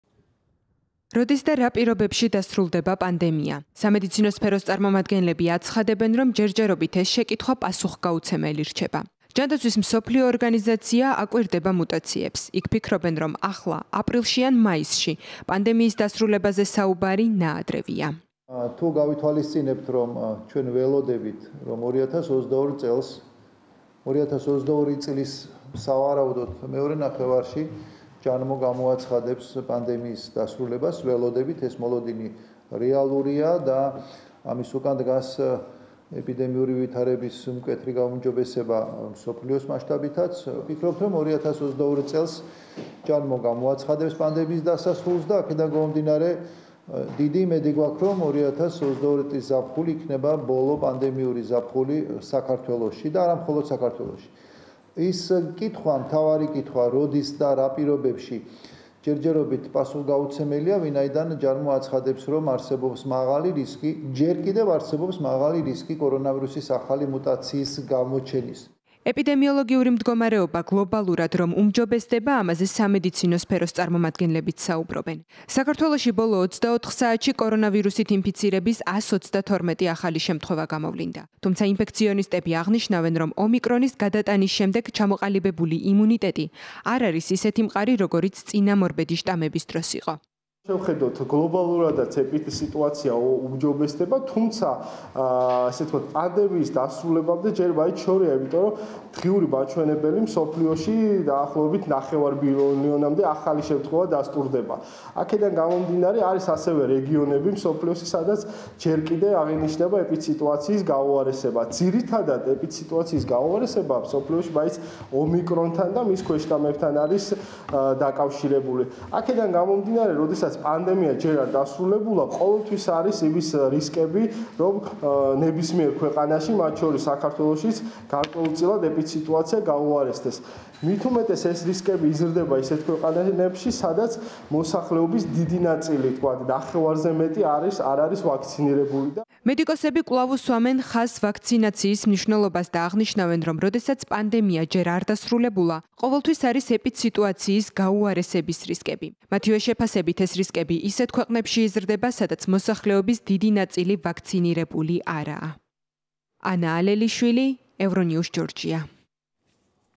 როდის და რა პირობებში დასრულდება პანდემია?
როდის და რა პირობებში დასრულდება პანდემია – სამედიცინო სფეროს წარმომადგენლები აცხადებენ, რომ ჯერჯერობით ეს შეკითხვა პასუხგაუცემელი რჩება. ჯანდაცვის მსოფლიო ორგანიზაცია აკვირდება მუტაციებს. იქ ფიქრობენ, რომ აპრილში ან მაისში პანდემიის დასასრულზე საუბარი ნაადრევია